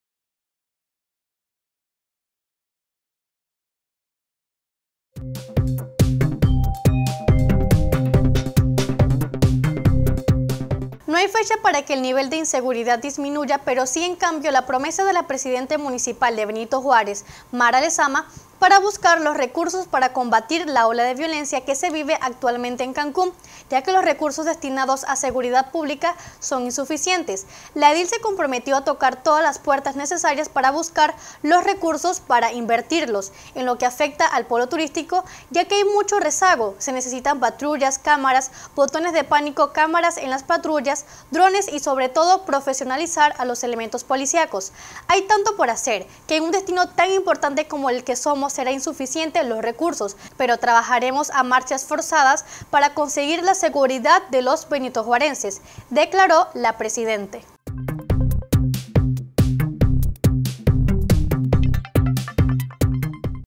NO HAY PLAZO PARA QUE NIVEL DE INSEGURIDAD DESCIENDA; MUCHO REZAGO EN SEGURIDAD PÚBLICA
Entérate de las noticias “EN CORTO”…NO HAY PLAZO PARA QUE NIVEL DE INSEGURIDAD DESCIENDA; MUCHO REZAGO EN SEGURIDAD PÚBLICA...